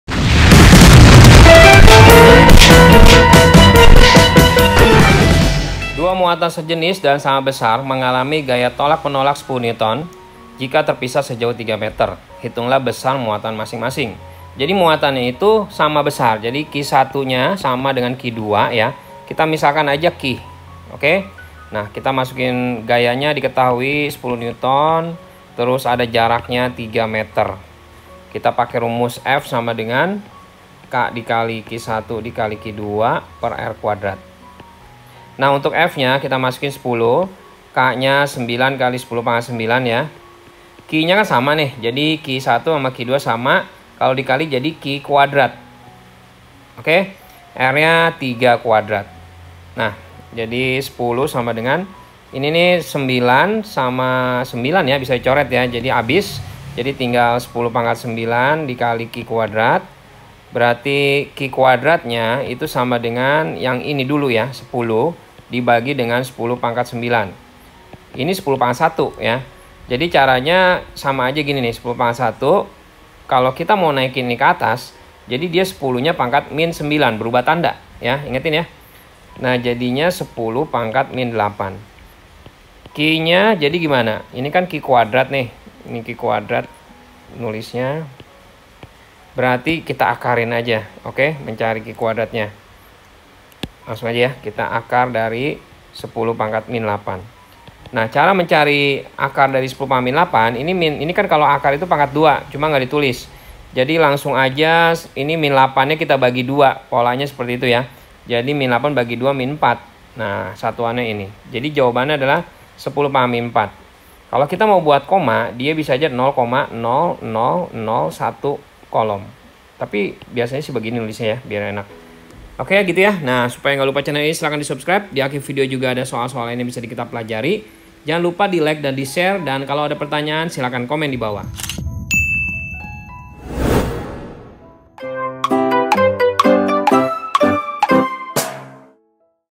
🔴LISTRIK STATIS🔴Dua muatan sejenis dan sama besar mengalami gaya tolak menolak 10 N jika terpisah se
Dua muatan sejenis dan sama besar mengalami gaya tolak menolak 10 n jika terpisah sejauh 3 m Hitunglah besar muatan masing-masing#matematikasmp#bilanganbulat#caramudah